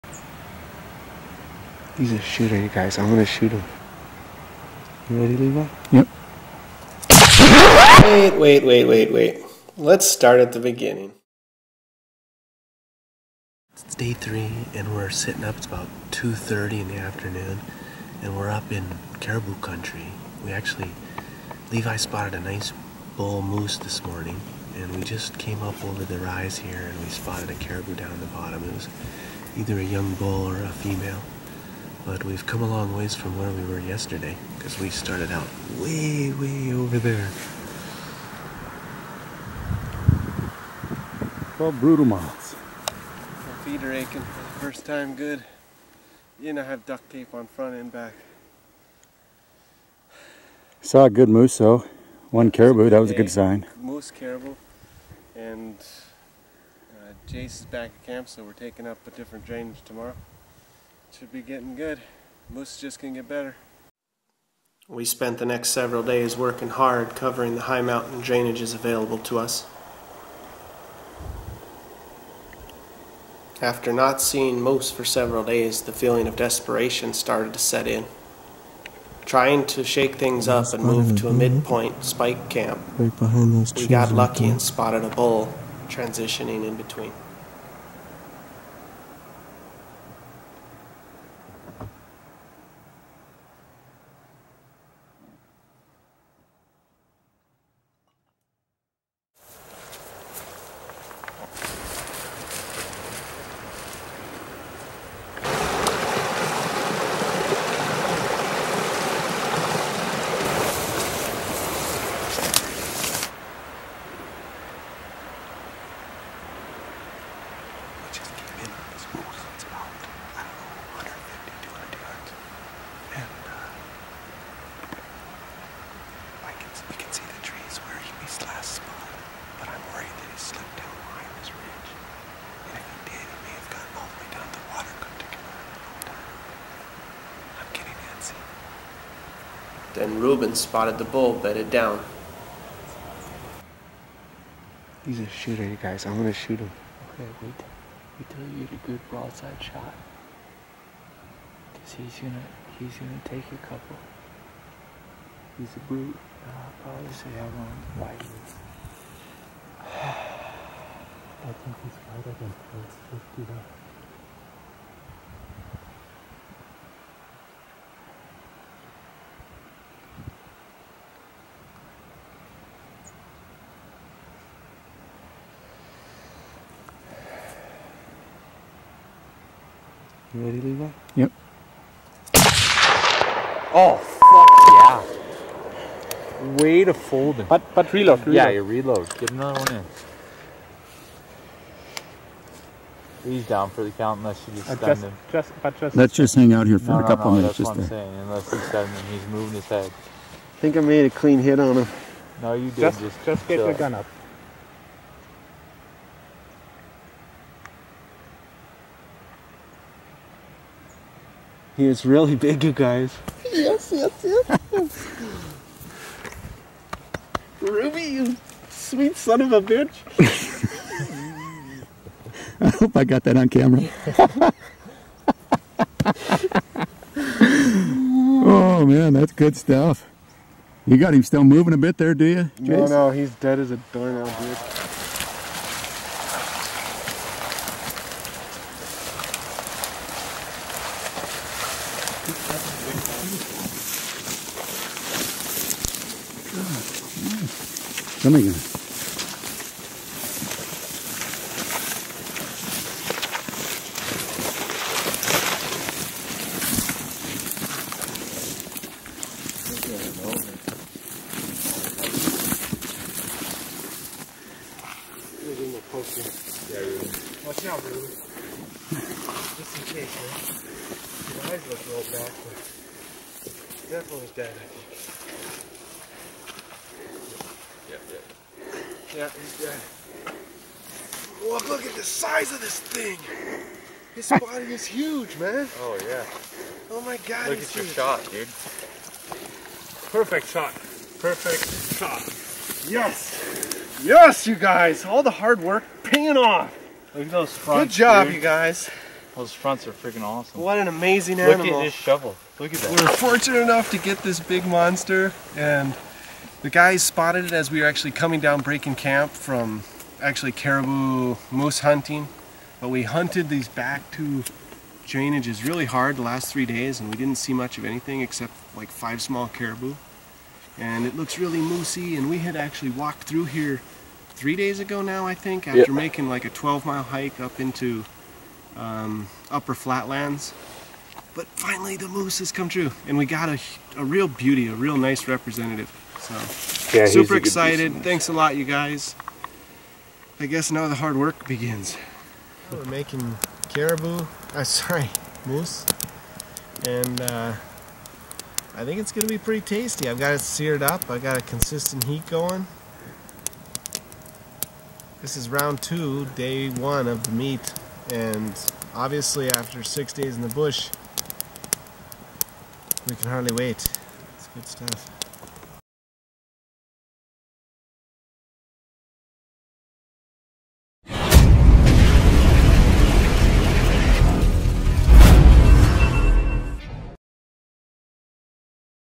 Early Season Canadian Moose Hunt
Wondering what it's like to go on a moose hunt in Northern British Columbia?  Follow Travis Volk hunting a record book Canadian Moose deep in the wilderness of British Columbia, Canada.
Produced by:  Adventure Mongers
Edited by:  Travis Volk
Camera operators:  Travis Volk and Levi Young